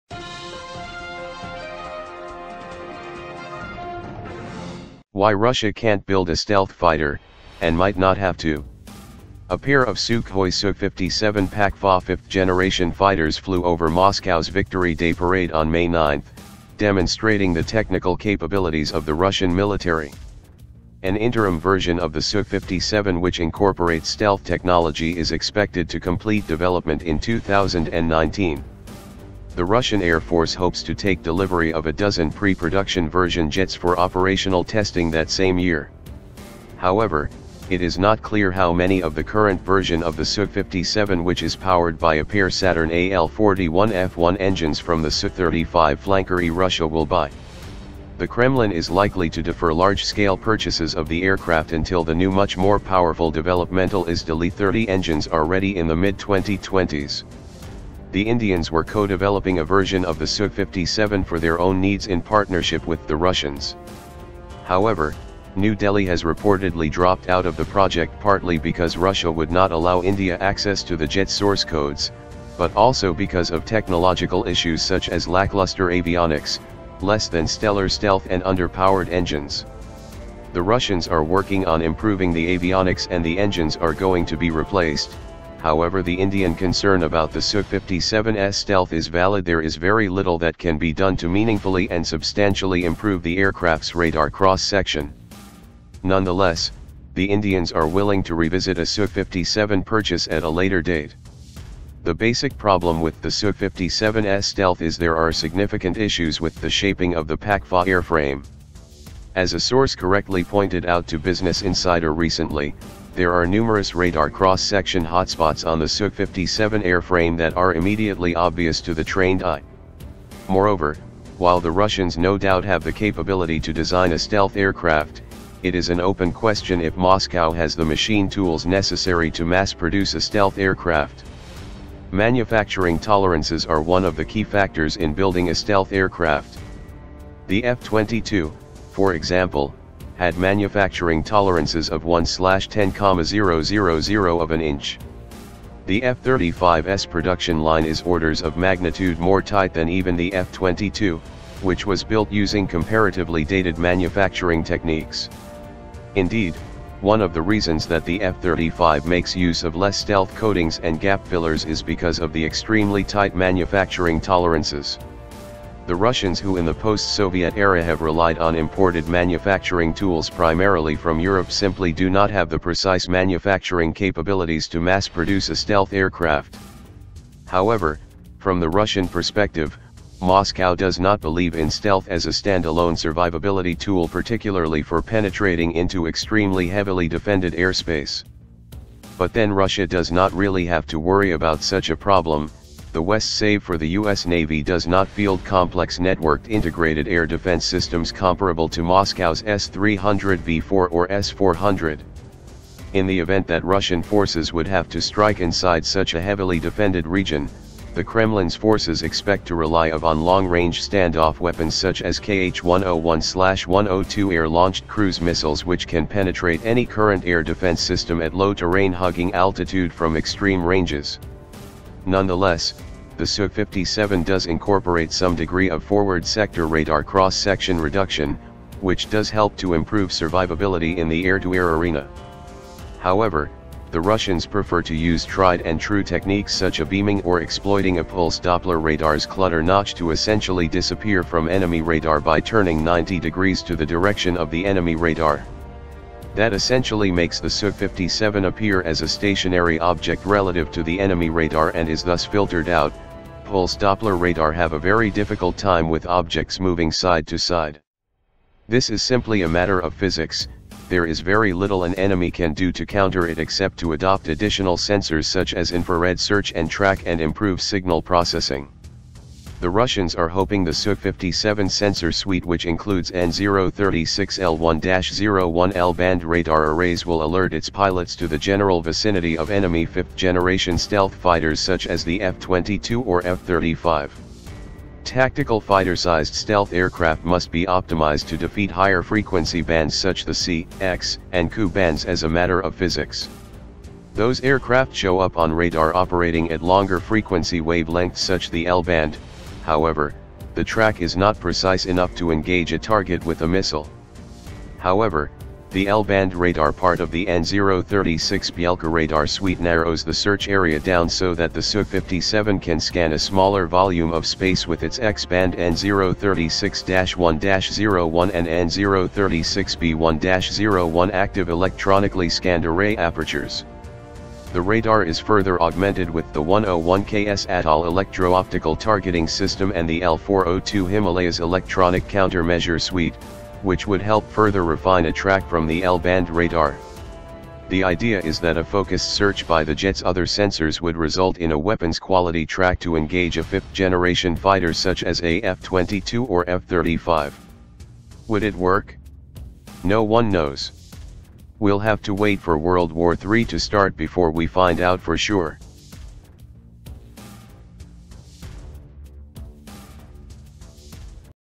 Why Russia Can't Build a Stealth Fighter (And Might Not Have To)
A pair of Sukhoi Su-57 PAK-FA fifth-generation fighters flew over Moscow’s Victory Day Parade on May 9, demonstrating the technical capabilities of the Russian military.

An interim version of the Su-57—which incorporates stealth technology—is expected to complete development in 2019. The Russian Air Force hopes to take delivery of a dozen pre production version jets for operational testing that same year. However, it is not clear how many of the current version of the Su-57—which is powered by a pair Saturn AL-41F1 engines from the Su-35 Flanker-E—Russia will buy. The Kremlin is likely to defer large-scale purchases of the aircraft until the new much more powerful developmental izdeliye 30 engines are ready in the mid-2020s.